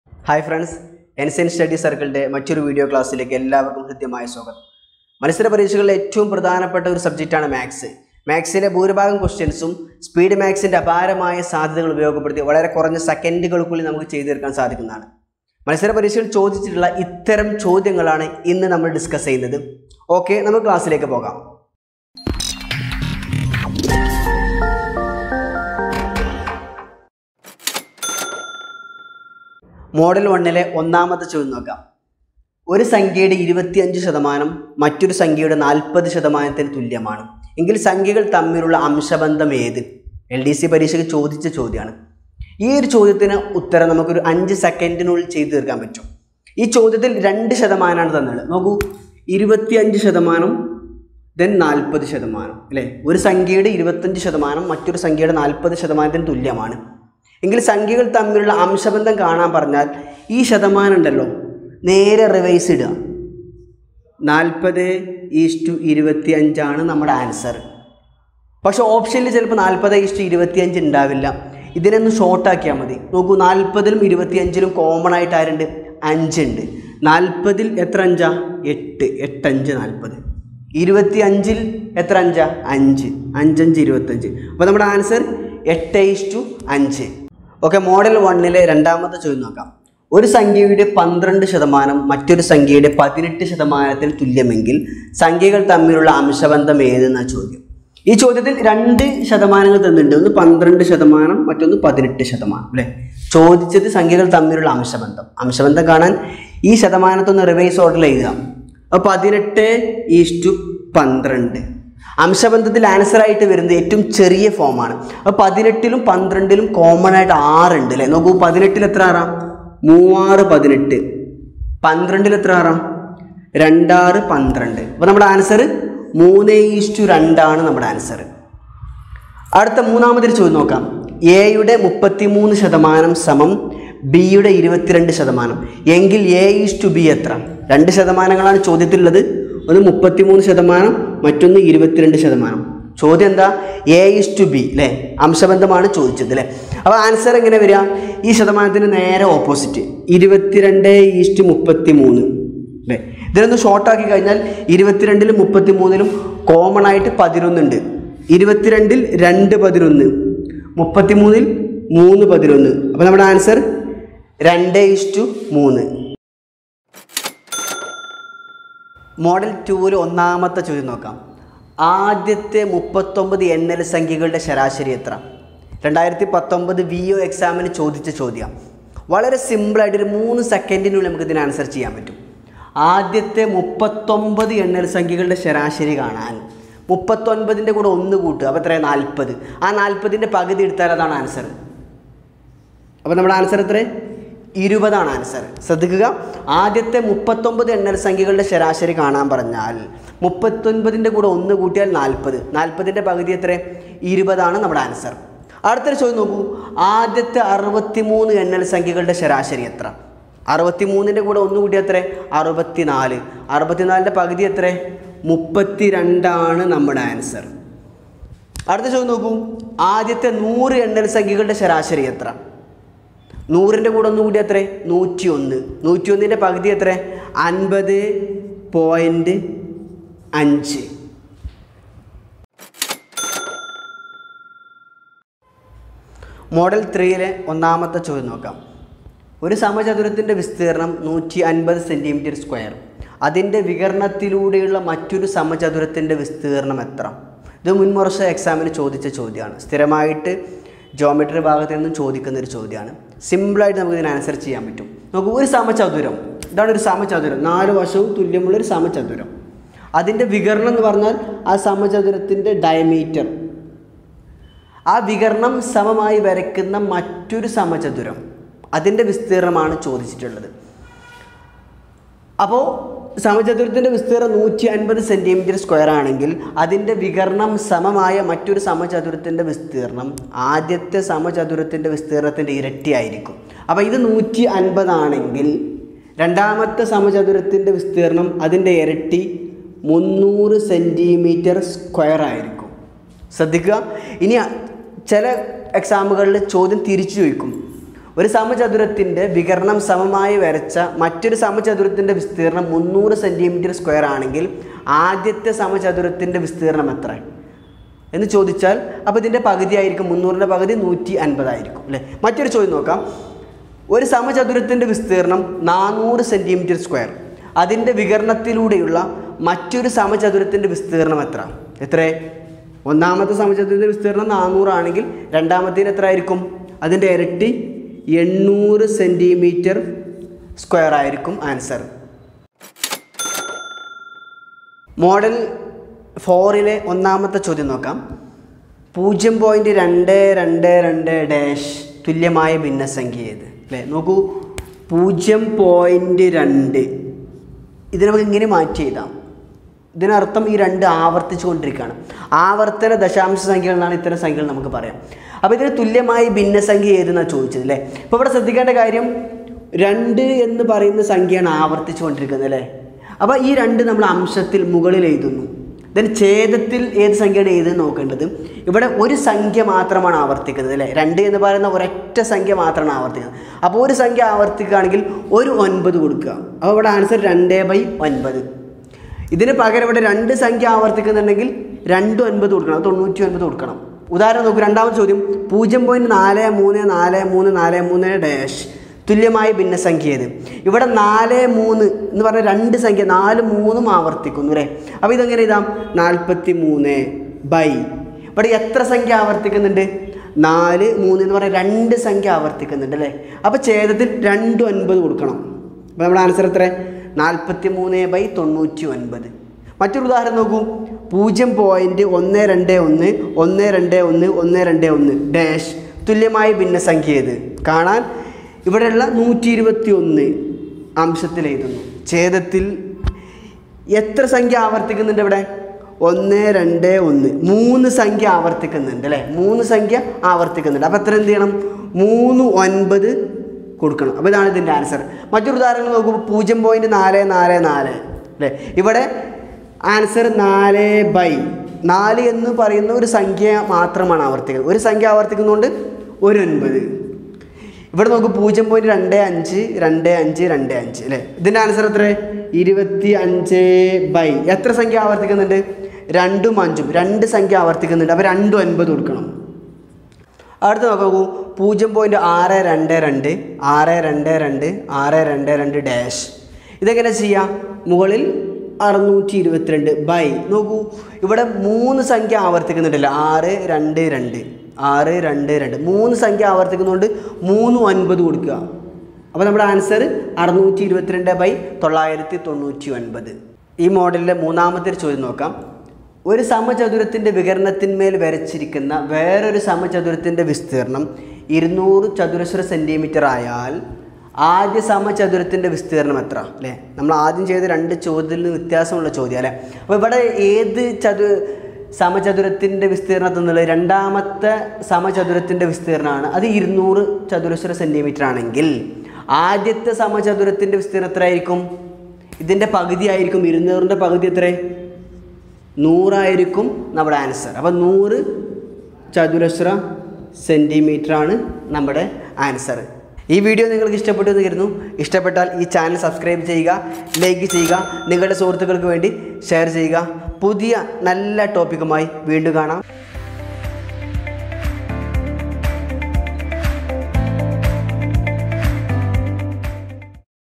PSC Maths Class Malayalam | PSC Maths Tricks | LDC SI LP/UP LGS | Previous Maths Questions
Here we are showing an interesting and simple method to solve PSC maths questions